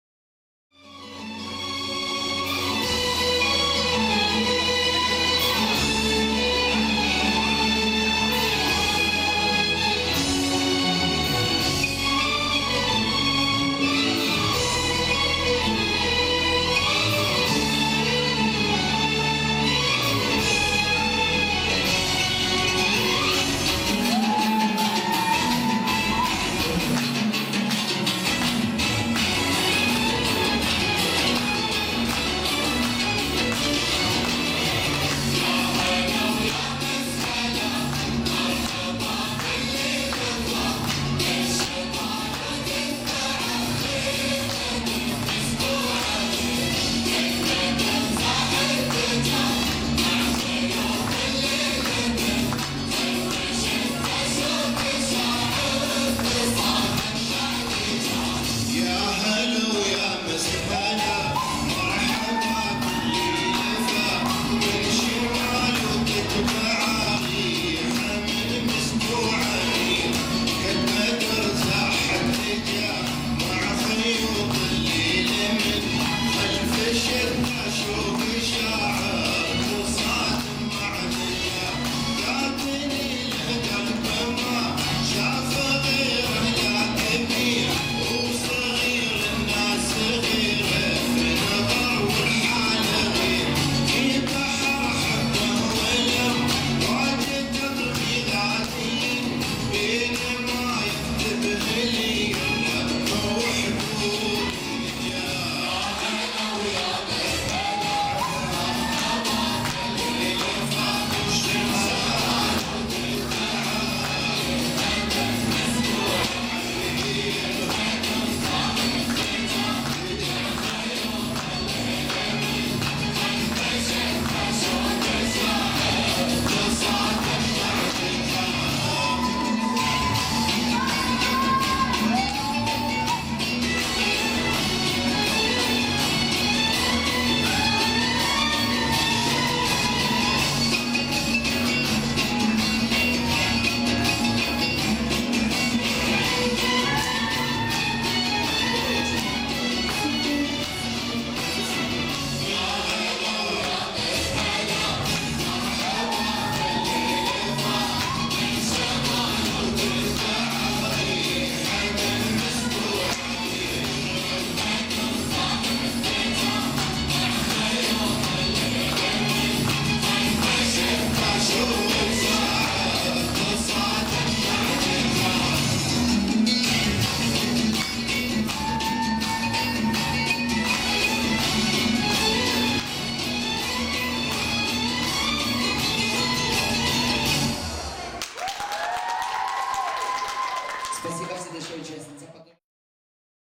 Maria Malinina ⊰⊱ Oriental Constellation '14.
Мария Малинина на конкурсе фестиваля "Созвездие Востока 2014".
02.11.14. Днепропетровск  ул. Шолом-Алейхема, 4/26. Центр «Менора»
(Фольклор Одна возрастная категория Соло Профессионалы)
 7,56